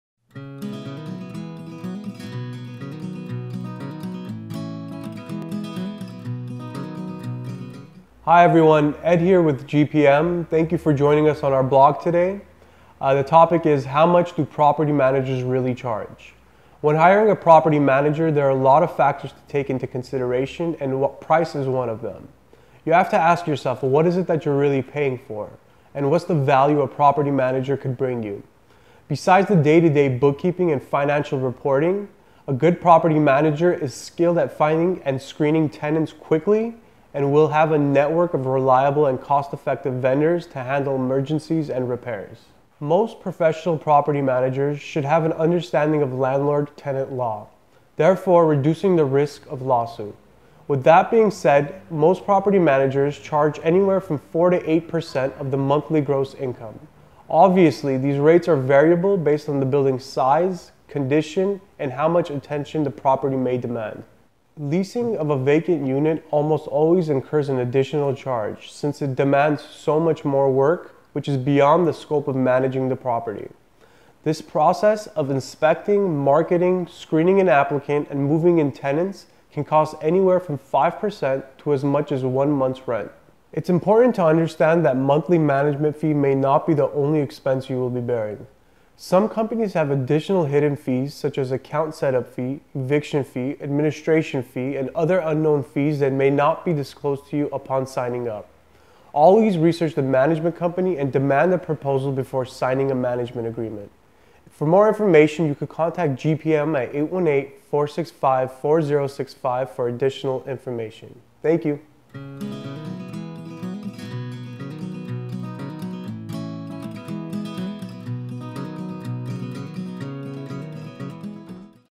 How Much Do Property Managers Really Charge? Hidden Fees in Glendale, CA
Today we’re talking about some of the things you should consider when hiring a property manager in Glendale. Price is important, and you will want to know what you’re really paying for, and what

1555 N Verdugo Rd Suite 101